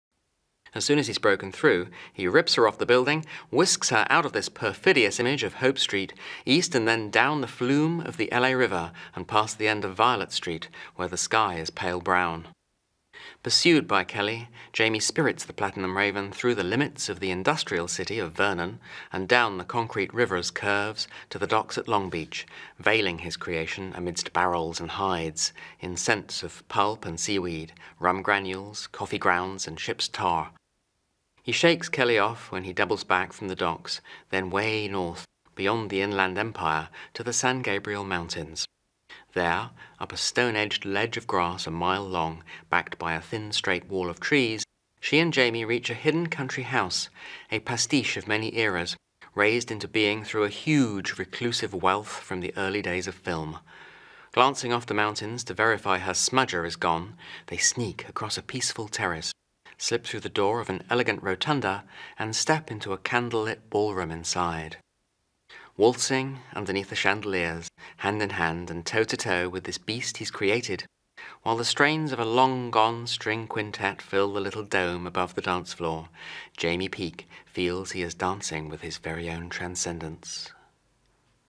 Teaser 74(ii), barrels and hides – for Rohan Quine’s “The Beasts of Electra Drive”
As soon as he’s broken through, he rips her off the building, whisks her out of this perfidious image of Hope Street, east and then down the flume of the L.A. River and past the end of Violet Street, where the sky is pale brown.

 Pursued by Kelly, Jaymi spirits the Platinum Raven through the limits of the industrial city of Vernon and down the concrete river’s curves to the docks at Long Beach, veiling his creation amidst barrels and hides, in scents of pulp and seaweed, rum granules, coffee grounds and ships’ tar. He shakes Kelly off when he doubles back from the docks, then way north, beyond the Inland Empire to the San Gabriel Mountains. There, up a stone-edged ledge of grass a mile long, backed by a thin straight wall of trees, she and Jaymi reach a hidden country house—a pastiche of many eras, raised into being through a huge reclusive wealth from the early days of film. Glancing off the mountains to verify her smudger is gone, they sneak across a peaceful terrace, slip through the door of an elegant rotunda, and step into a candle-lit ballroom inside.

 Waltzing underneath the chandeliers, hand in hand and toe to toe with this Beast he created, while the strains of a long-gone string quintet fill the little dome above the dance-floor, Jaymi Peek feels he is dancing with his very own transcendence…